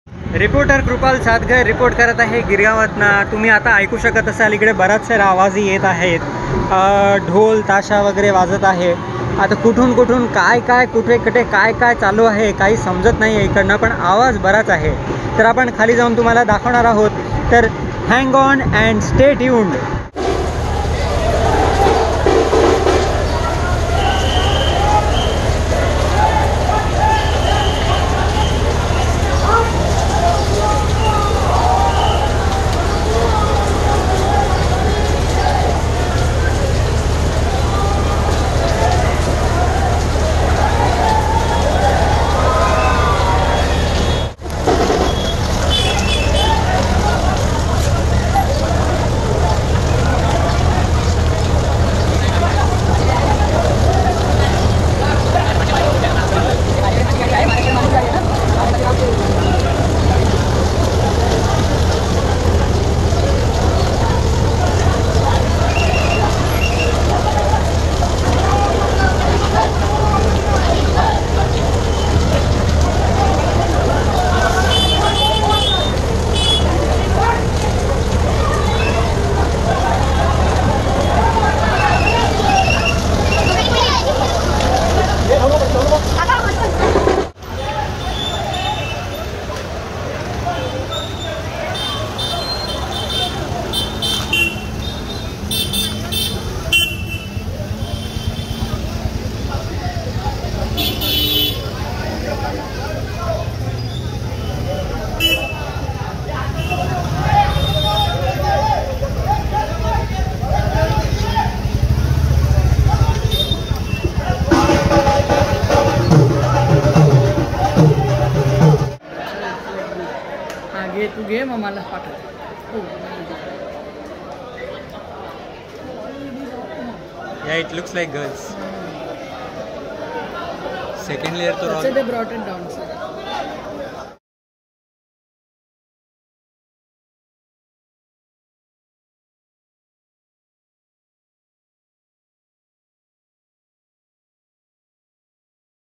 on occasion of krishnajanmashtami I was in girgaon to see dahihandi had superfun checkout some
on the occasion of krishnajanmashtami I was in girgaon to see dahihandi had
check out some of the govinda